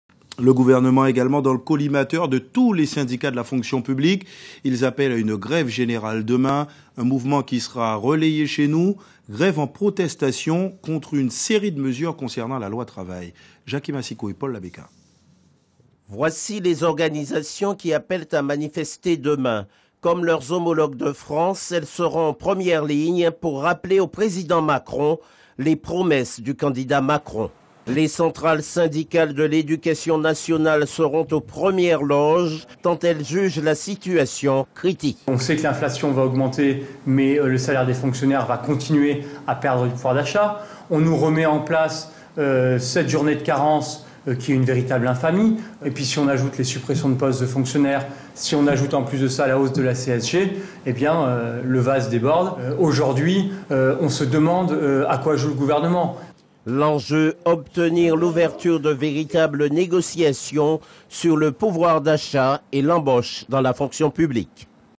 Pourquoi faire grève le 10 octobre
Interview de Guillaume Marsault, secrétaire académique du SNES-FSU Guadeloupe, par Guadeloupe 1ère concernant les raisons de la grève Fonction Publique du 10 Octobre.